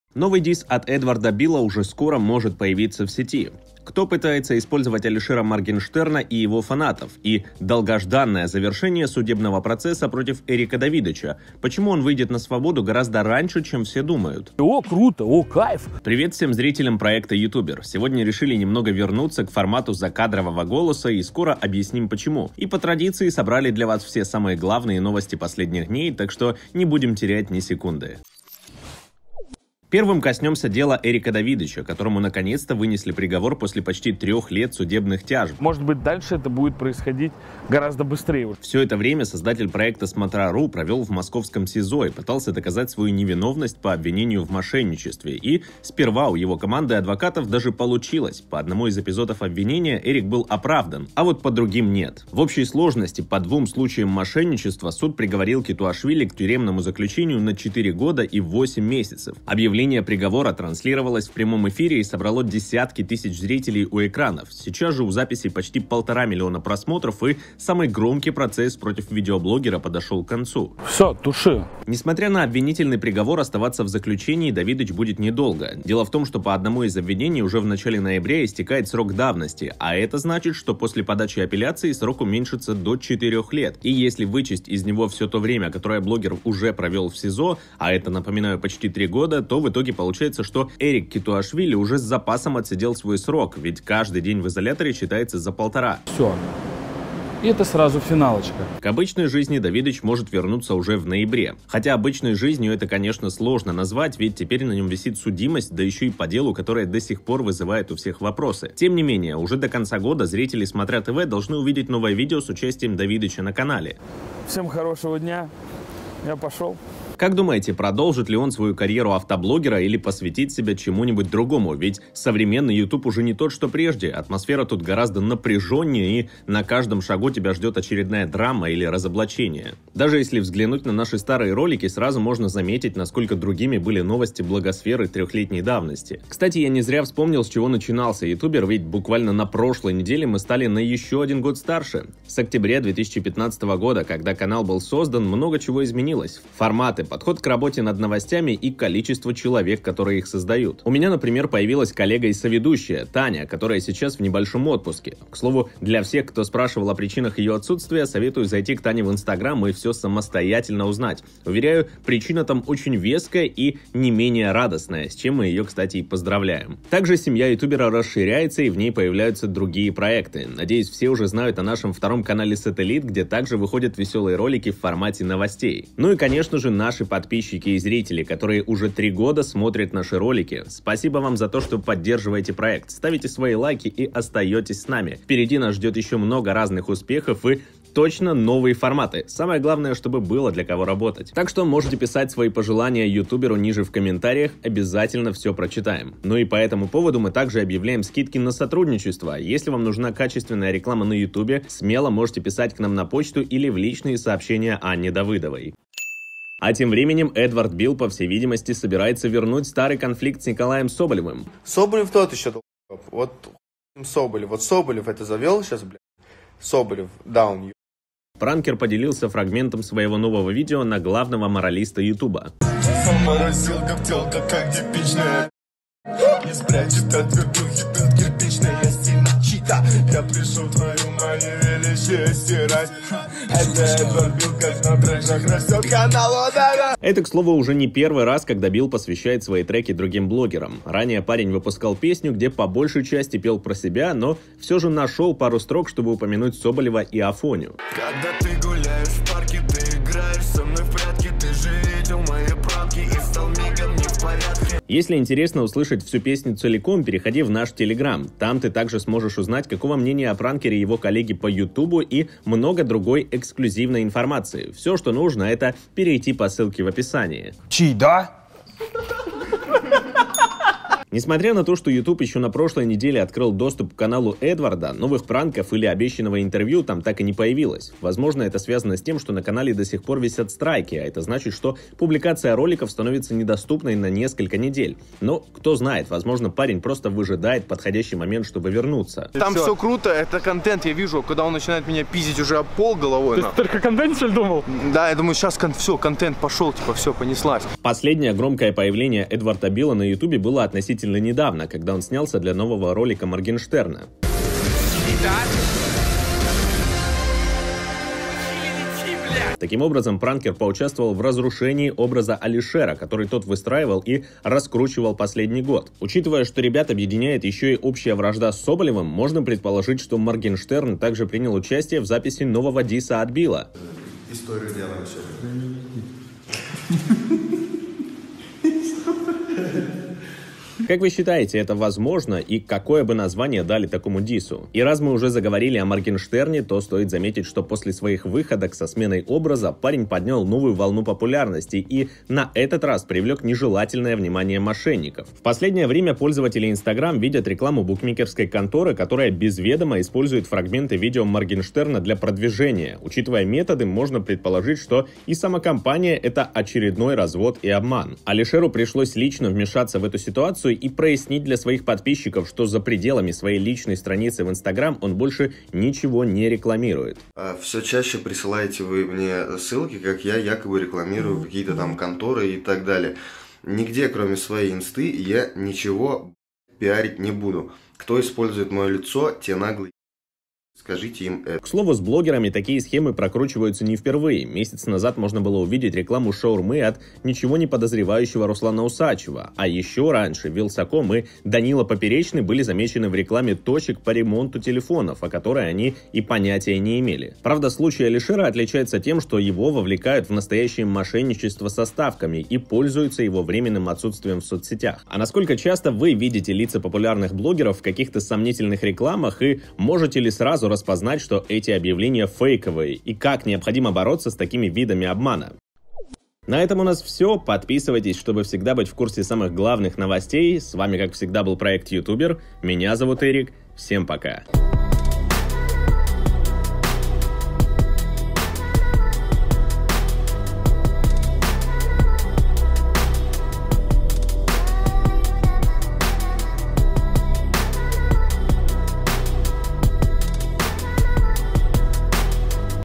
ЭДВАРД БИЛ ЗАПИСАЛ ДИСС НА СОБОЛЕВА! МОРГЕНШТЕРН - ЖЕРТВА МОШЕННИКОВ
Эдвард Бил по всей видимости собирается вернуть старый конфликт с Николаем Соболевым. Пранкер поделился фрагментом своего нового дисса на главного морализатора Ютуба.

Это к слову не первый раз, когда Бил посвящает треки другим блогерам. Ранее парень уже выпускал песню, где по большей части пел про себя, но все же нашел пару строк чтобы упомянуть Соболева и Афоню.

Несмотря на то, что Ютуб еще на прошлой неделе открыл доступ к каналу Эдварда, новых пранков или обещанного интервью там так и не появилось. Возможно это связано с тем, что на канале до сих пор висят страйки, а это значит, что публикация роликов становится недоступной на пару недель. Но кто знает, возможно, парень просто выжидает подходящий момент, чтобы вернуться.

Последнее громкое появление Эдварда Била на ютубе было относительно недавно, когда он снялся для нового ролика Моргенштерна.

Таким образом пранкер поучаствовал в разрушении образа Алишера, который тот выстраивал и раскручивал последний год. Учитывая, что ребят объединяет еще и общая вражда с Соболевым, можем предположить, что Моргенштерн также принял участие в записи нового дисса от Била.